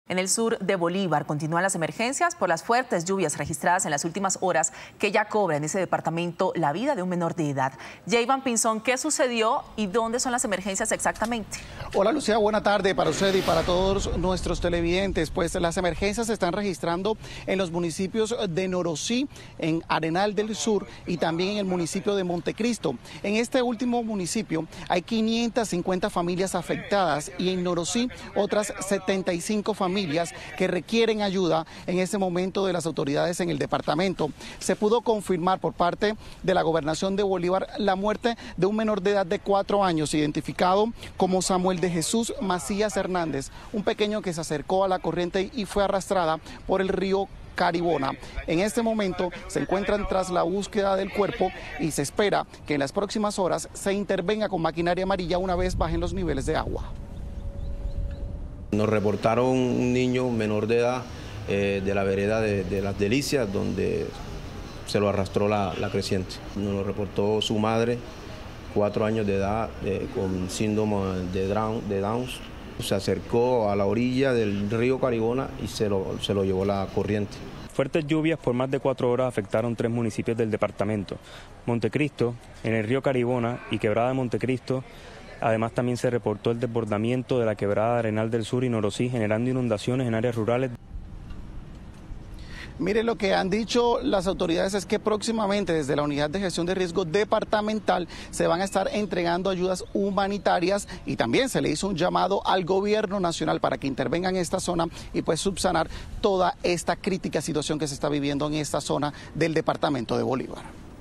Niño de 4 años muere tras ser arrastrado por una corriente en Bolívar
En el sur de Bolívar continúan las emergencias por las fuertes lluvias registradas en las últimas horas y que ya cobran la vida de un menor de edad.